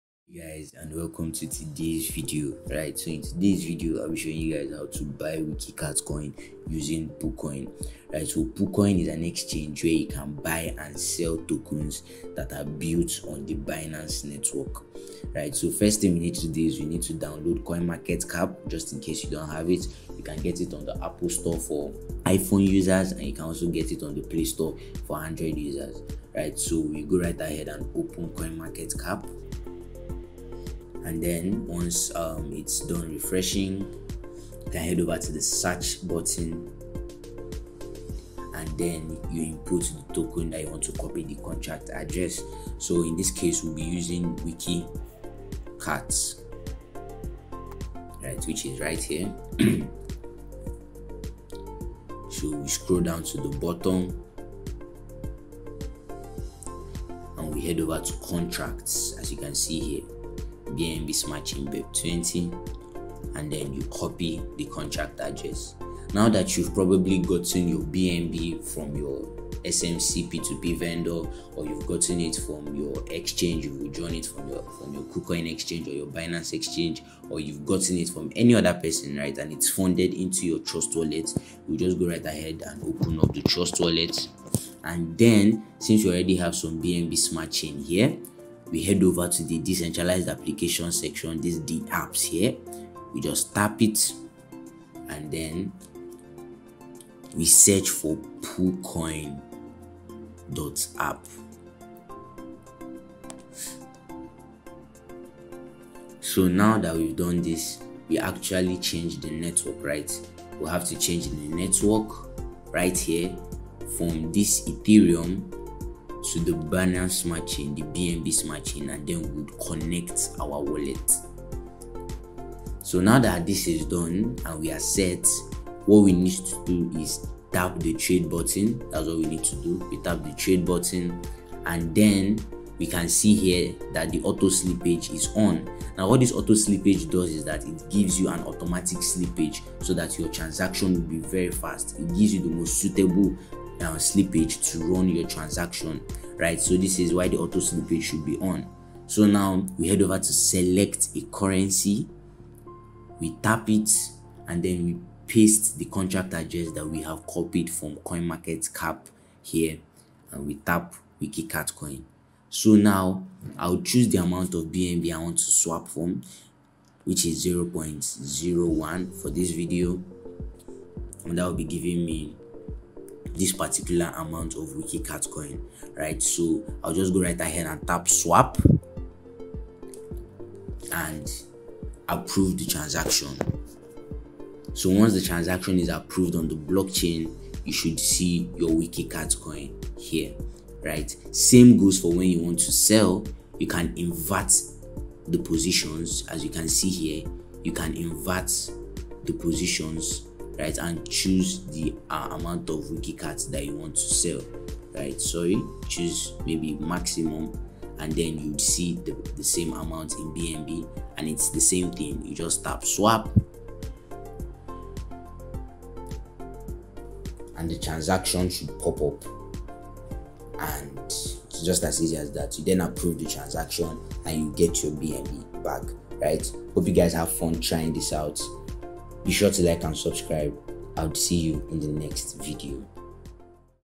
How to buy Wiki Cat using BNB on Trust Wallet via Poocoin.app
(00:00) Introduction to the tutorial on buying Wiki Cat Coin using PooCoin.
(00:04) Overview of PooCoin as an exchange for tokens on the Binance network.
(00:09) Instructions to download Coin Market Cap from the Apple Store or Play Store.
(00:15) Opening Coin Market Cap and searching for Wiki Cat Coin.
(00:27) Copying the contract address of Wiki Cat Coin from Coin Market Cap.
(00:51) Scrolling down to the contracts section to find the Binance Smart Chain (BSC) address.
(01:06) Acquiring BNB from an exchange or other sources.
(01:11) Funding BNB into Trust Wallet.
(01:22) Opening Trust Wallet and accessing the decentralized application section.
(01:31) Searching for PooCoin.app in Trust Wallet.
(01:37) Changing the network from Ethereum to Binance Smart Chain.
(01:51) Connecting the wallet to PooCoin.
(02:02) Using the trade button on PooCoin.
(02:07) Explanation of auto slippage for faster transactions.
(02:29) Selecting a currency and pasting the Wiki Cat Coin contract address.
(02:36) Choosing the amount of BNB to swap for Wiki Cat Coin.
(02:49) Swapping BNB for Wiki Cat Coin.
(02:57) Approving the transaction.
(03:04) Confirmation of the transaction on the blockchain.
(03:08) Viewing Wiki Cat Coin in the wallet.
(03:14) Instructions for selling Wiki Cat Coin.
(03:21) Inverting positions for selling.
(03:26) Choosing the amount of Wiki Cat Coin to sell.
(03:32) Swapping Wiki Cat Coin for BNB.
(03:40) Approving the sell transaction.
(03:45) Conclusion and encouragement to try out the process.
(03:50) Invitation to like and subscribe to the channel.